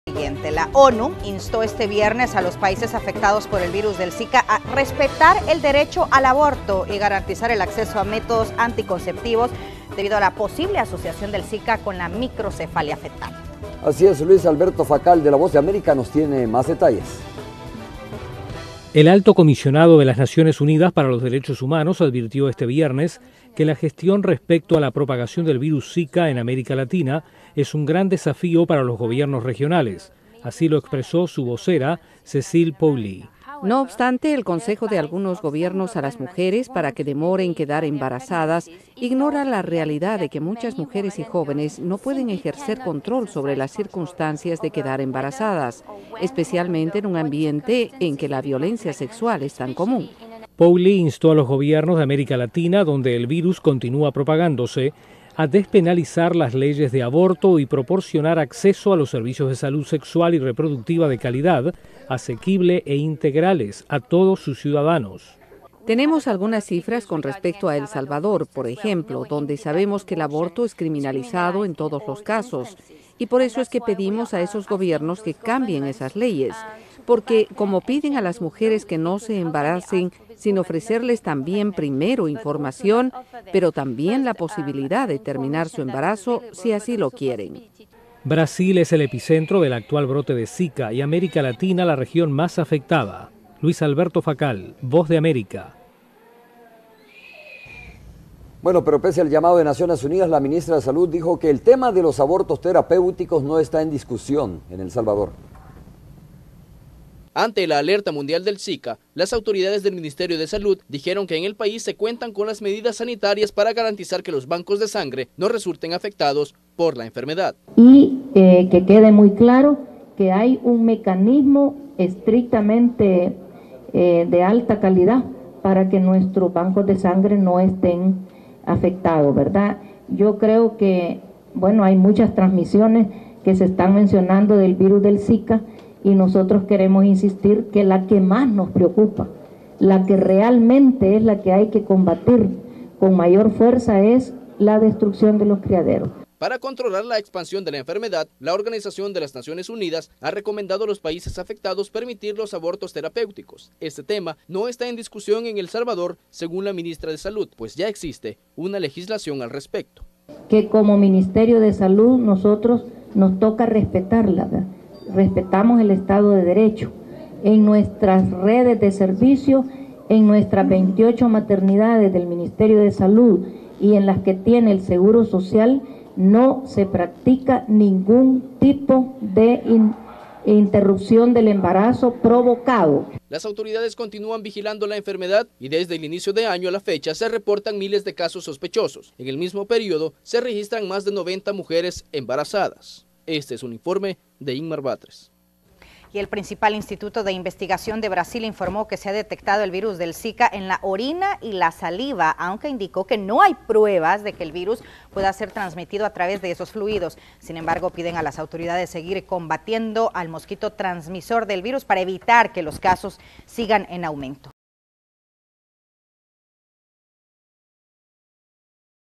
ONU promueve respetar el derecho al aborto
La Organización de Naciones Unidas instó a los países afectados por el Zika a respetar el derecho al aborto y garantizar el acceso de métodos anticonceptivos.